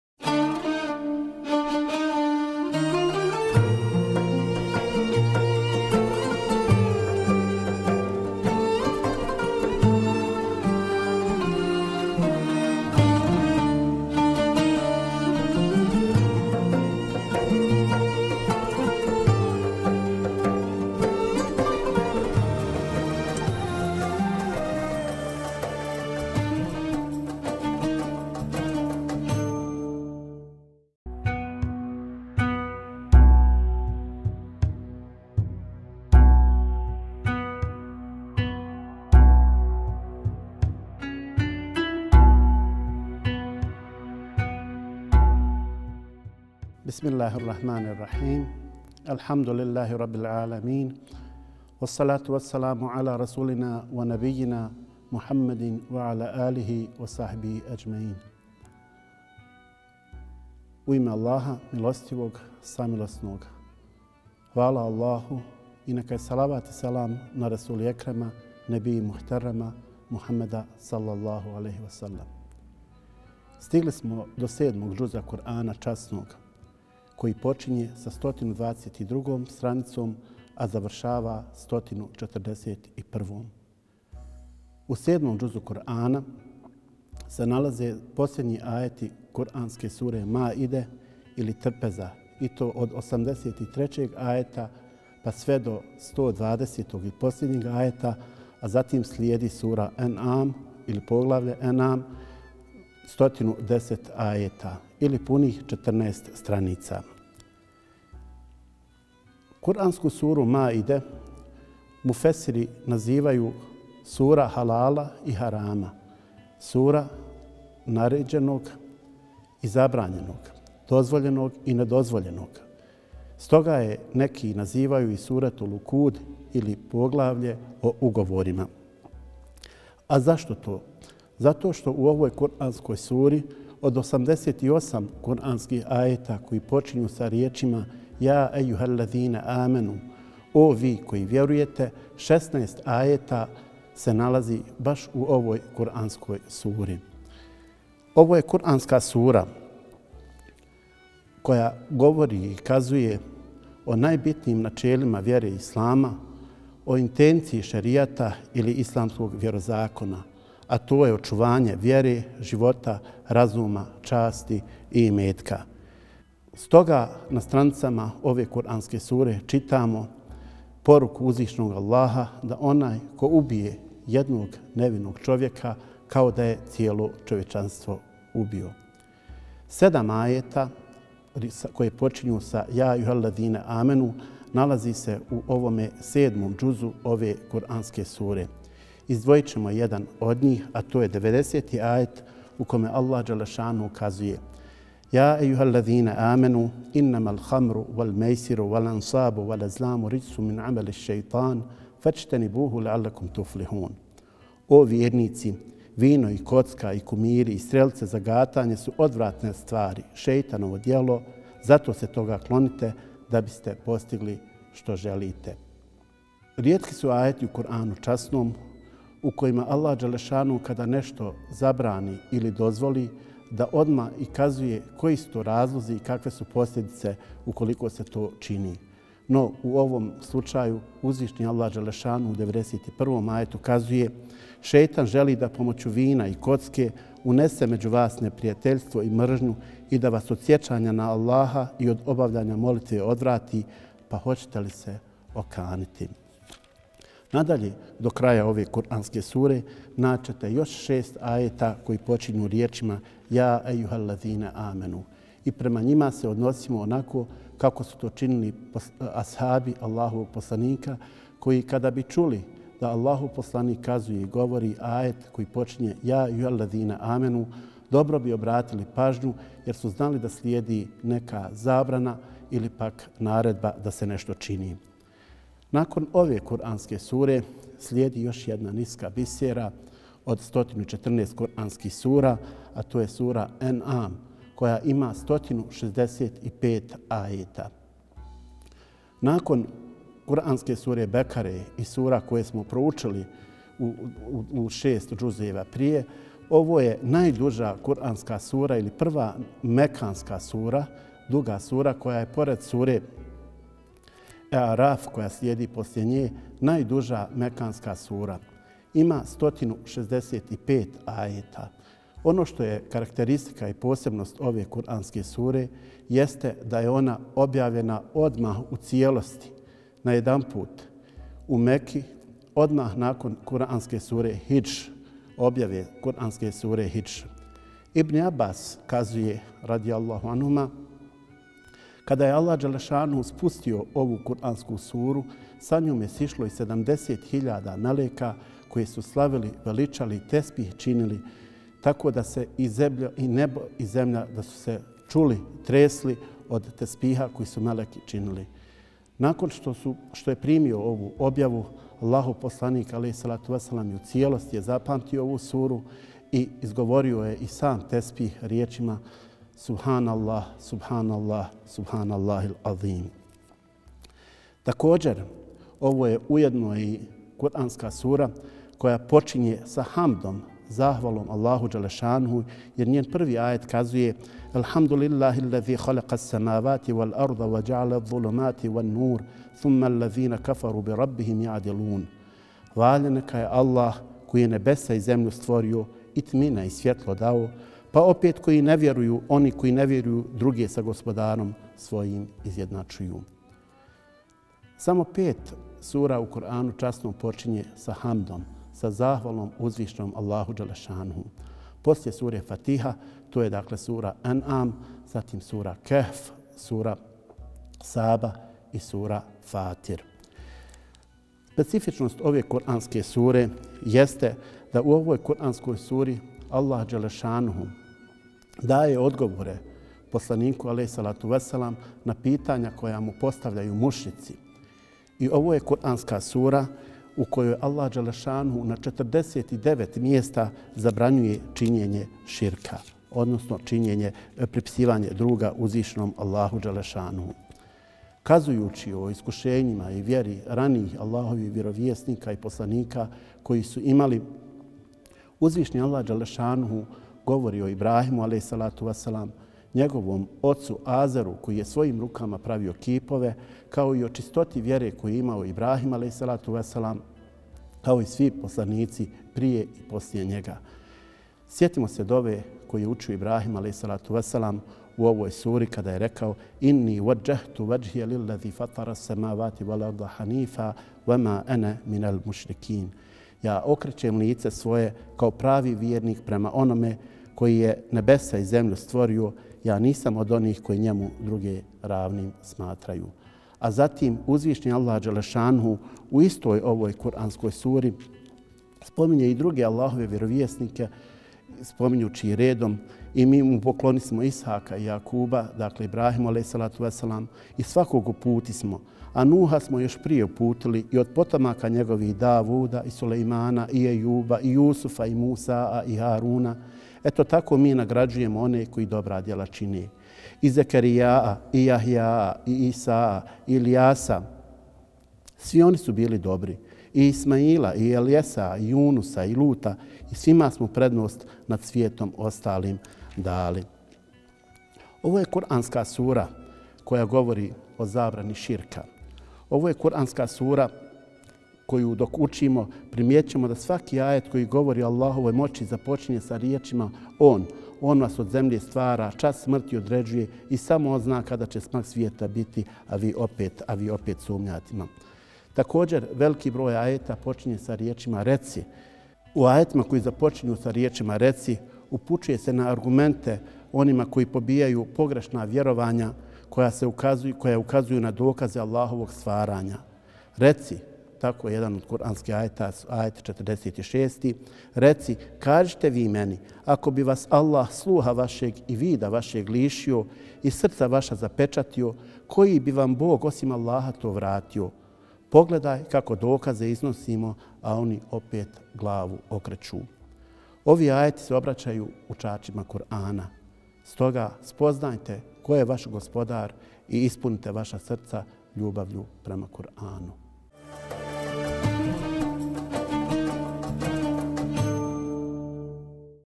Komentar 7. džuza Kur'ana: Muslija-ef. Čeliković
Muslija-ef. Čeliković, imam Centralne džamije u Vogošći, Medžlis Islamske zajednice Sarajevo, komentira 7. džuz iz Kur'ana Časnog, donoseći osnovne poruke sura i ajeta koji se u njemu nalaze.